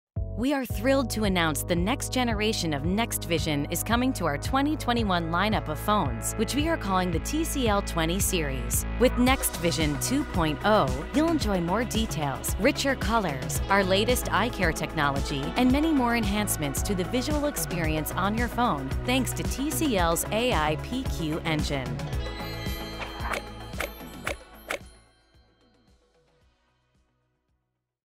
Introducing NXTVISION 2.0
TCL 20 Series phones feature our next generation of NXTVISION, our proprietary advanced display optimization and camera technology that debuted with the TCL 10 Series last year.